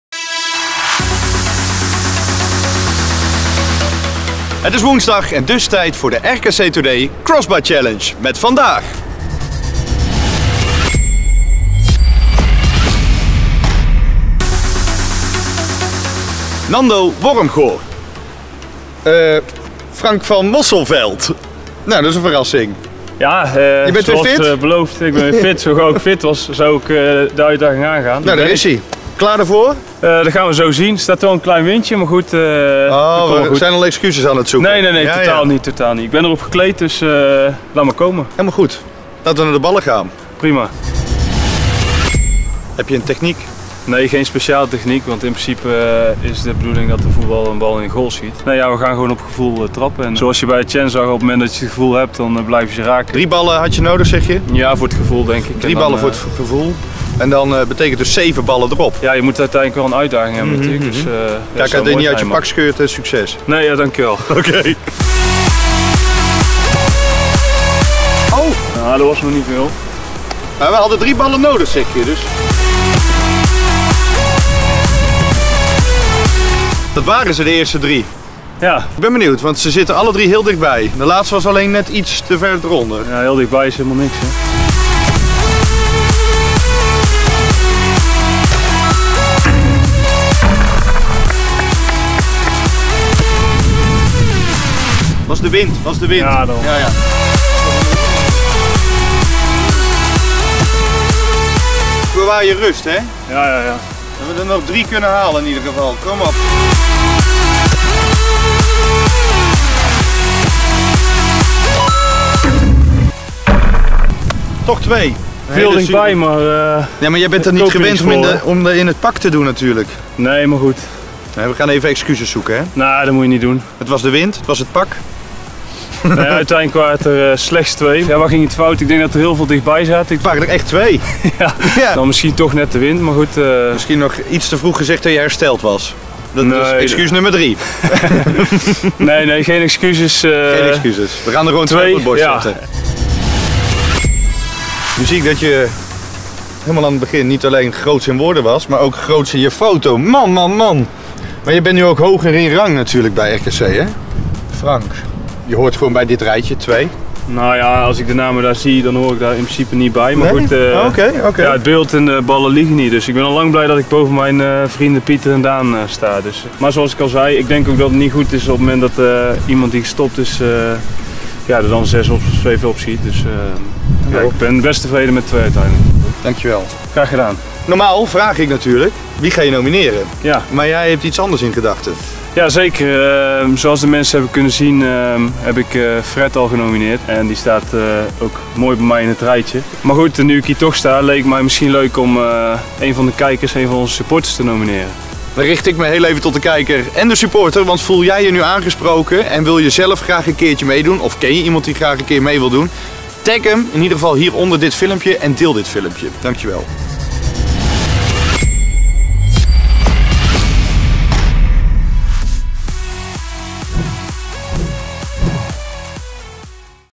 RKC Today Crossbar Challenge 10 Frank van Mosselveld
Etienne Vaessen, de nieuwe nr 1 in de crossbar challenge, nomineerde Nando Wormgoor. Er stond echter nog een rekening open van oud-speler Frank van Mosselveld, die na zijn blessure een poging waagt in de RKC Today crossbar Challenge!